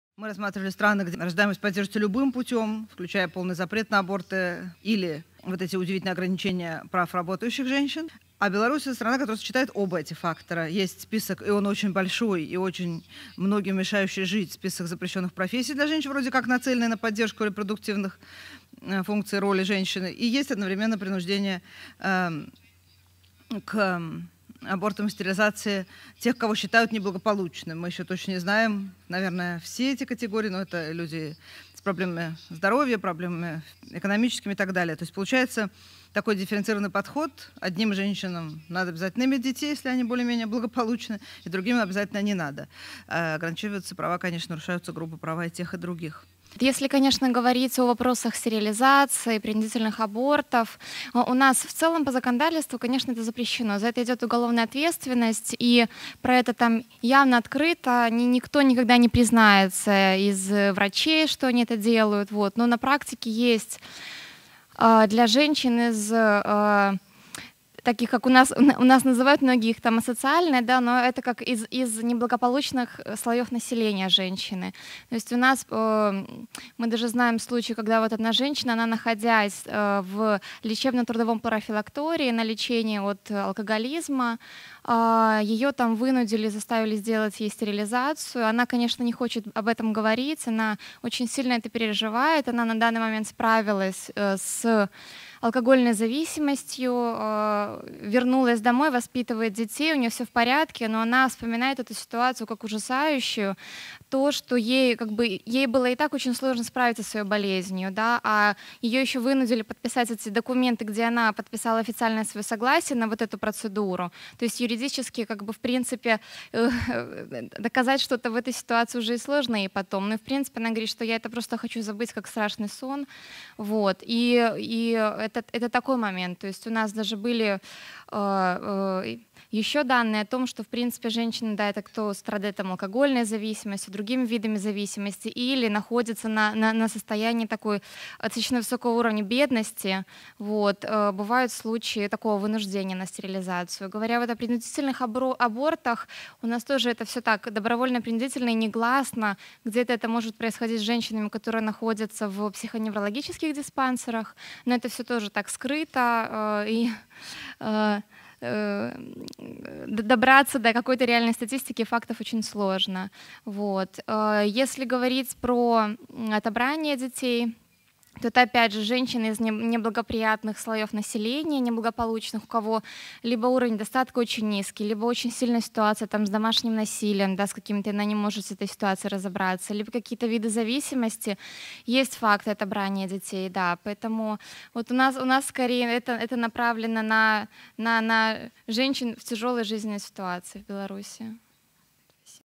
Беларусь. Нарушения репродуктивных прав женщин в постсоветских странах
Александра Дикан, центр "Её права" (Беларусь), сообщает, что несмотря на запреты принудительных абортов и стерилизации, такие случаи происходят. В особенности от этого страдают женщины из неблагополучных слоев населения. Известны случаи, когда женщин, находящихся на профилактическом лечении от алкоголизма, вынуждали сделать стерилизацию. Разлучение детей также происходит, как правило среди неблагополучных семей, однако такая практика может применяться и в качестве репрессивной меры в отношении женщин.